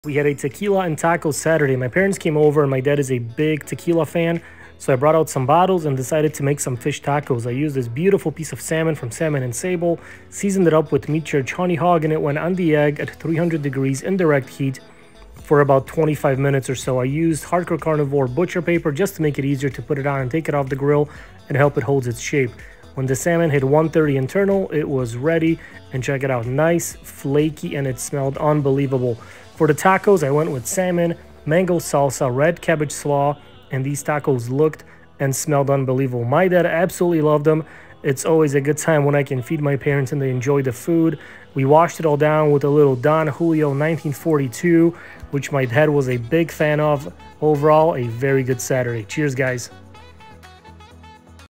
Tequila and Tacos
Tacos and tequila from yesterday. For the tacos, I used a beautiful piece of salmon from @salmonandsable . I seasoned it with @meatchurch Honey Hog and it hit the @biggreenegg at 300 degrees indirect heat. Once it hit 130 internal, it was ready to go. ⠀